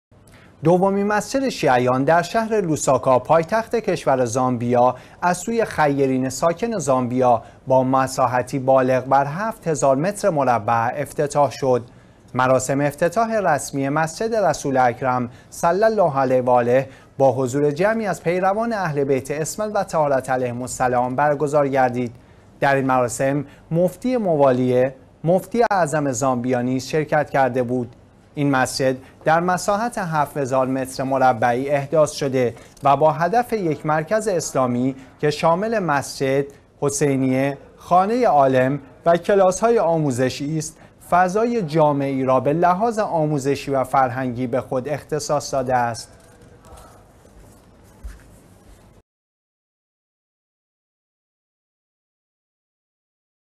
افتتاح دومین مسجد شیعیان در پایتخت زامبیا
دومین مسجد شیعیان در شهر لوساکا پایتخت کشور زامبیا از سوی خیرین ساکن زامبیا با مساحتی بالغ بر هفت هزار متر مربع افتتاح شد. ‌
مراسم افتتاح رسمی مسجد رسول اکرم صلی الله علیه و آله با حضور جمعی از پیروان اهل بیت عصمت و طهارت علیهم السلام برگزار شد.
در این مراسم مفتی موالیه، مفتی اعظم زامبیا نیز شرکت کرده بود.
این مسجد در مساحت ۷ هزار متر مربعی احداث شده و با هدف یک مرکز اسلامی که شامل مسجد، حسینیه، خانه عالم و کلاس های آموزشی است فضای جامعی را به لحاظ آموزشی و فرهنگی به خود اختصاص داده است.